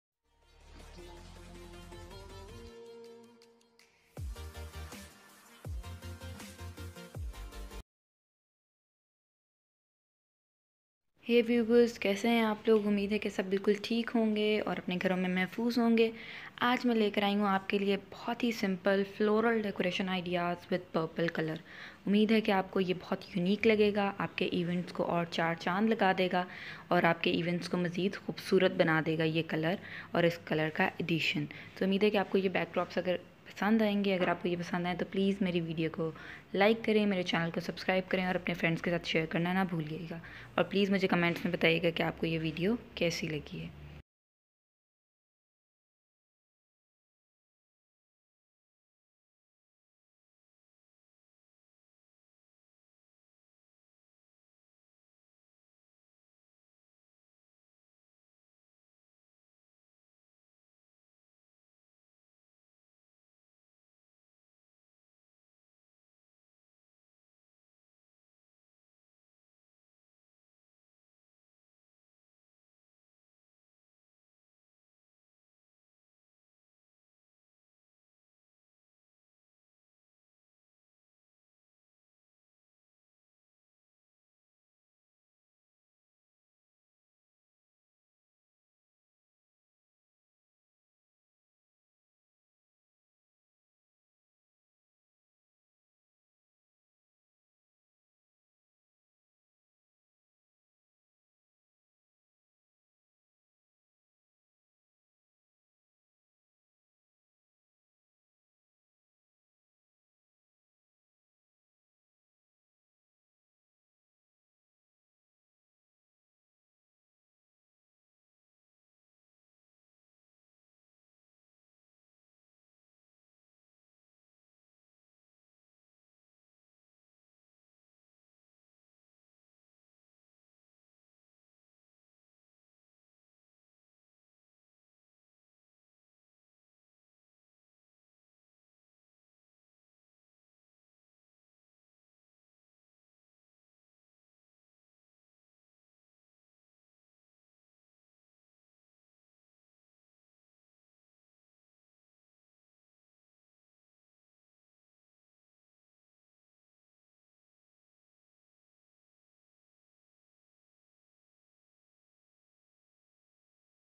Backdrop decoration ideas with purple colour - Simple and easy backdrop Decoration ideas
Hello Friends
In this video to you will get simple and easy purple colour backdrop decoration ideas.

backdropdecor ideas
decorwith purple